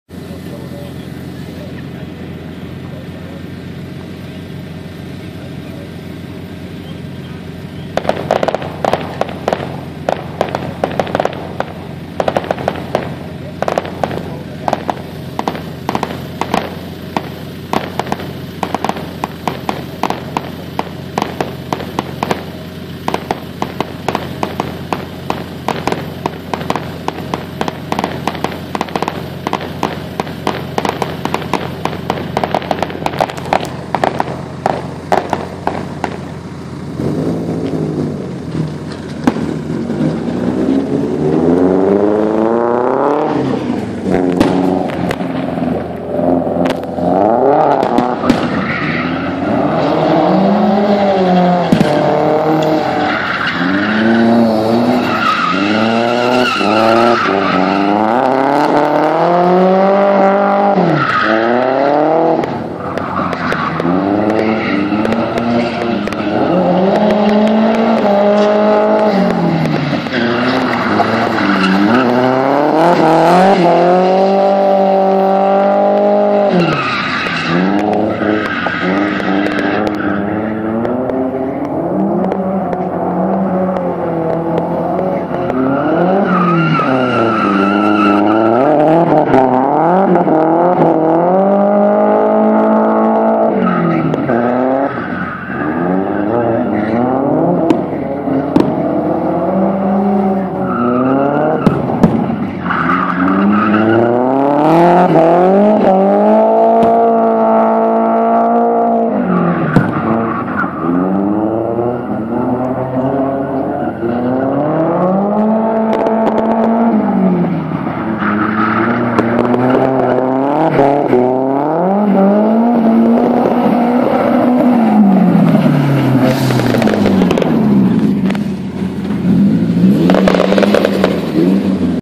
Santapod fireworks final 2012 with drifting
Little vid of fireworks and some skids, sparks flying :-)